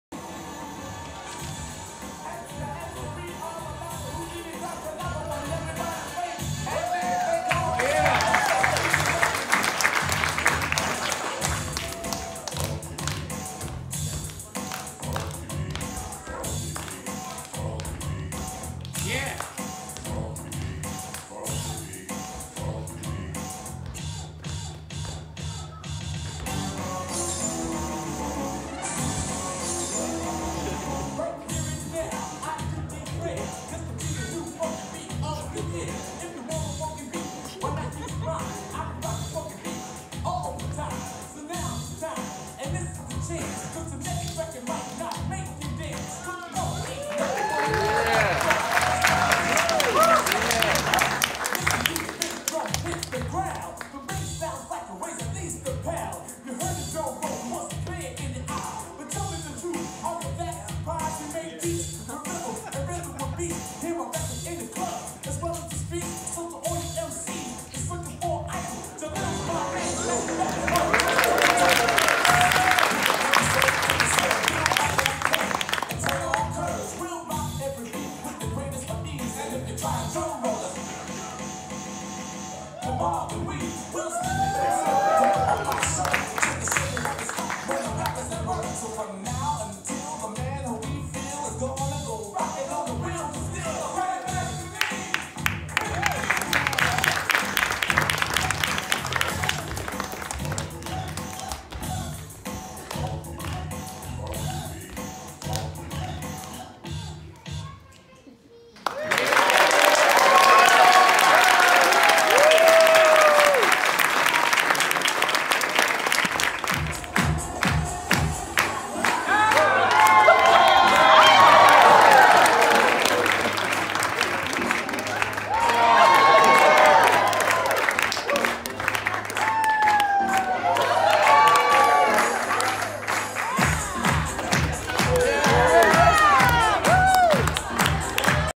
5 Year Old Lui First Dance Performance ルイのダンス発表会
今年から始めたヒップホップクラスでの初めての発表会です。決められた振り付けよりも、フリースタイルの方が好きみたい